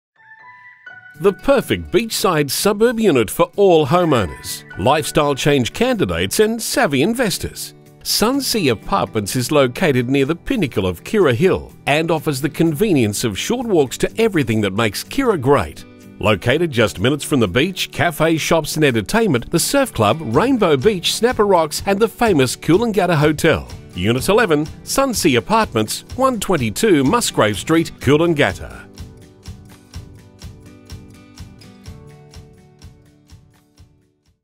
11/122 Musgrave Street, Coolangatta QLD 4225
The perfect beach side suburb unit for all homeowners, lifestyle change candidates and savvy investors. Sunsea apartments is located near the pinnacle of Kirra Hill and offers the convenience of short walks to everything that makes Kirra great. Located just minutes from the beach, cafes, shops and entertainment, the surf club, Rainbow Beach, Snapper Rocks and the famous Coolangatta Hotel!Unit 11 Sunsea Apartments, 122 Musgrave Street, Coolangatta